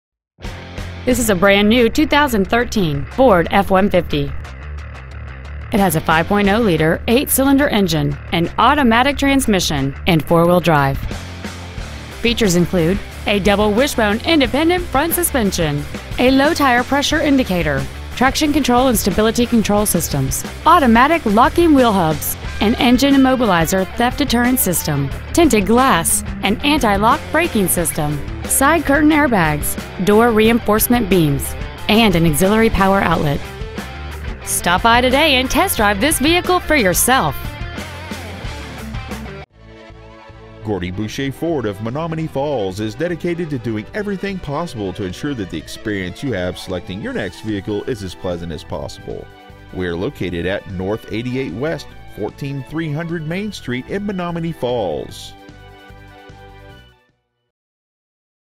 2013 FORD F-150 Menomonee Falls WI
Year : 2013
 Make : FORD
 Model : F-150
 Engine : 5.0L V8 32V MPFI DOHC FLEXIBLE FUEL
 Trans . : 6-SPEED AUTOMATIC
 Exterior : GREEN GEM METALLIC
 Miles : 10
 Interior : BLACK
 Stock : 13F162